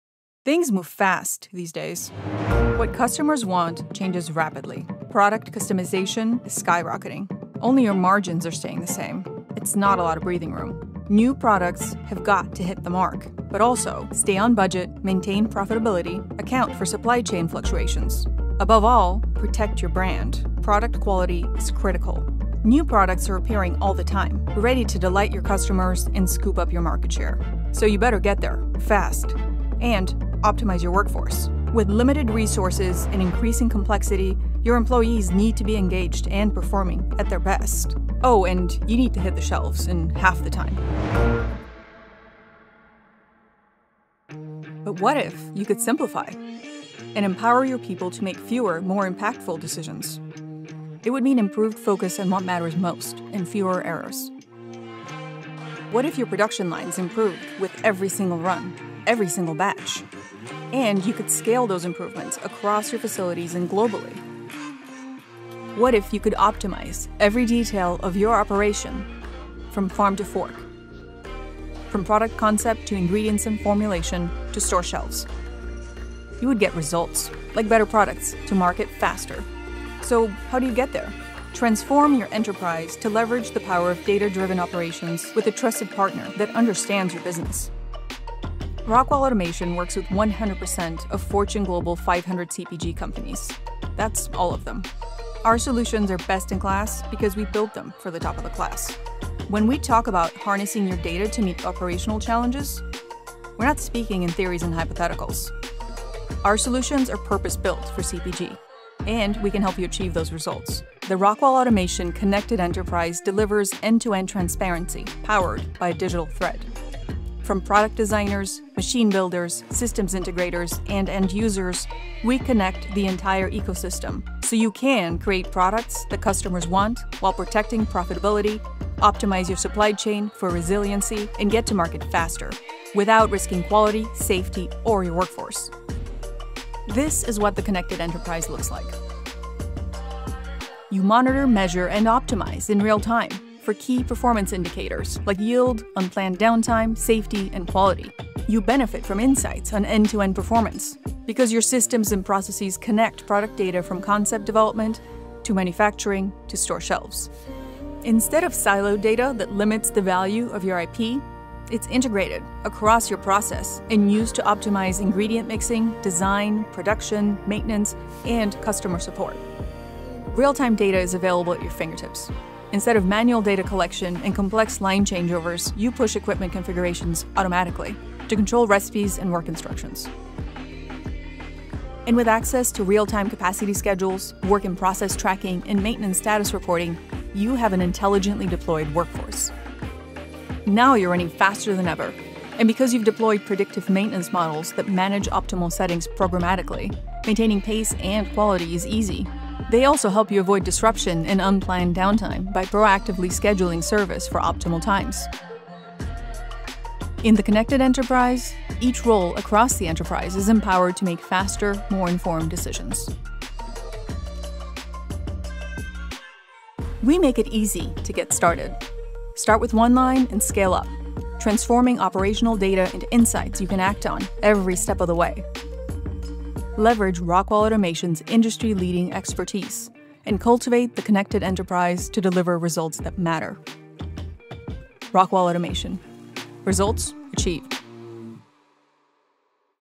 CPG Knowledge-Driven Operations: Results Achieved
See how Rockwell Automation can harness your data to meet the operational challenges presented by a complex CPG industry.